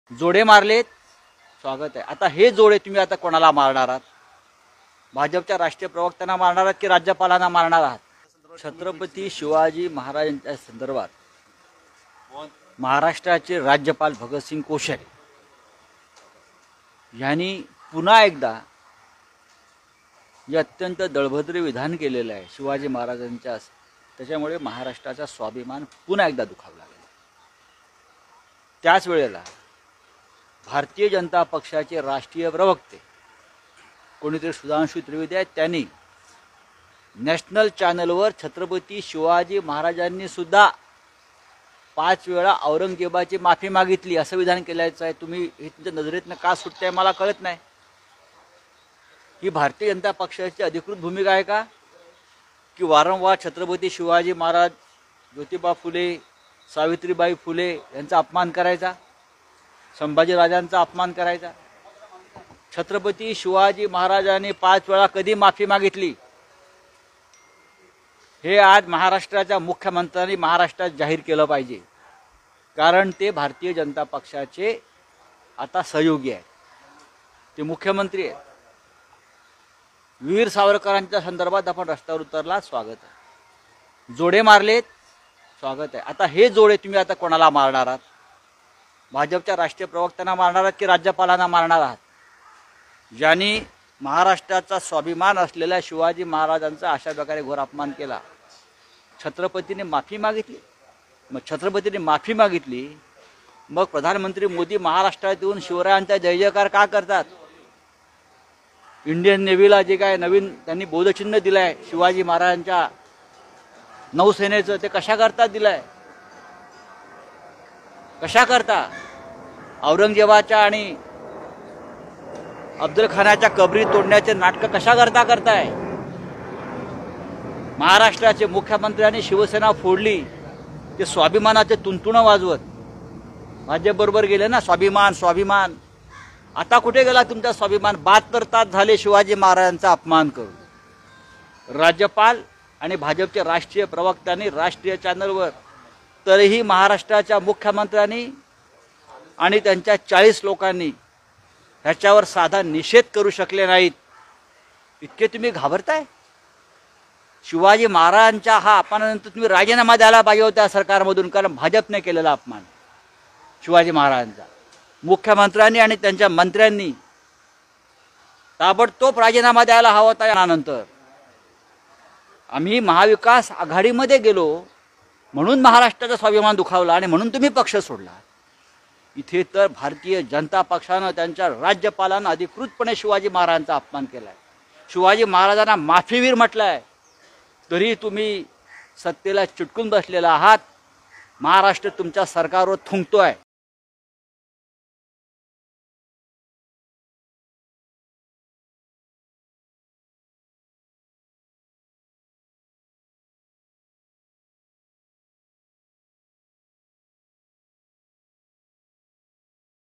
Sanjay Raut On Maharashtra Government : 'ते जोडे' राज्यपालांना मारणार का? संजय राऊतांचा सवाल
राज्यपाल भगतसिंह कोश्यारी आणि भाजपाचे राष्ट्रीय प्रवक्ते सुधांशु त्रिवेदी यांनी छत्रपती शिवाजी महाराज यांच्याबाबत वादग्रस्त वक्तव्य केले आहे. याच मुद्द्यावरून शिवसेना खासदार संजय राऊत यांनी सरकारला धारेवर धरले आहे.